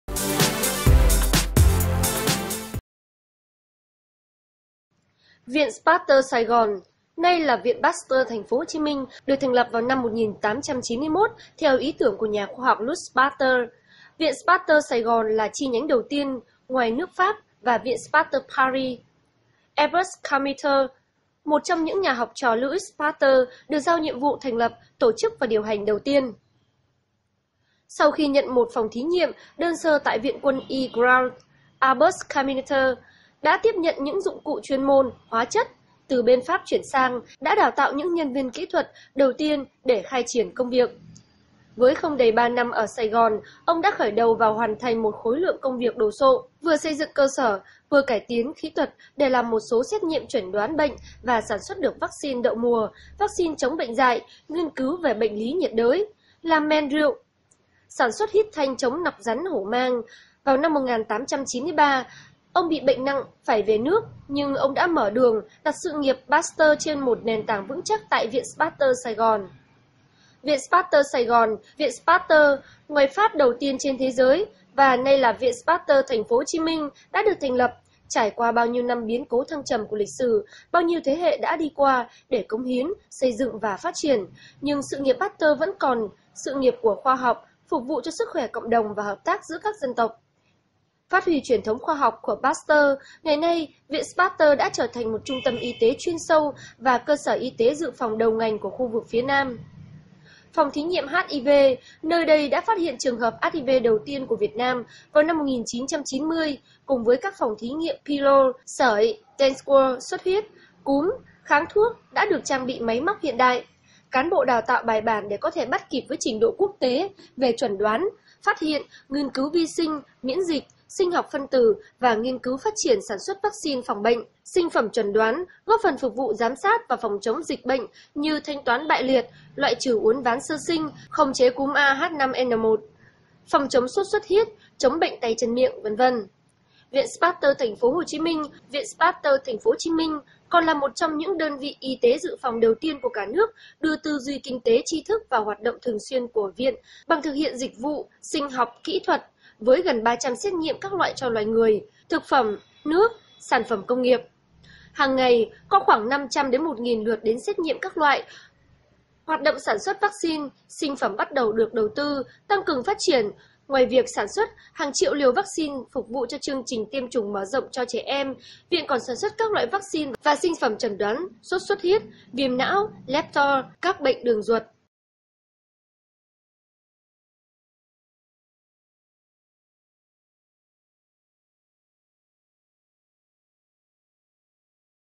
Chiêm ngưỡng kiến trúc Viện Pasteur trăm năm tuổi ở Sài Gòn
Tin báo mới hôm nay Chiêm ngưỡng kiến trúc Viện Pasteur trăm năm tuổi ở Sài Gòn. Viện Pasteur Sài Gòn nay là Viện Pasteur TP HCM đã có lịch sử hơn 100 năm tuổi...